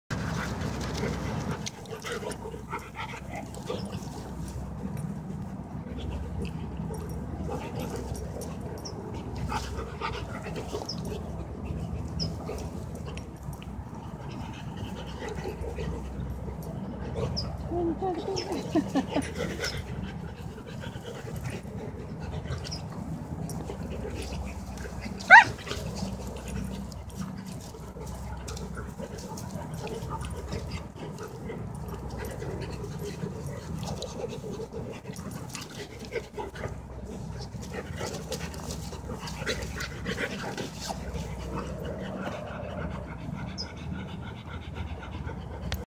Labrador Moby Playing with Retriever Billy
Moby playing with his best friend Billy.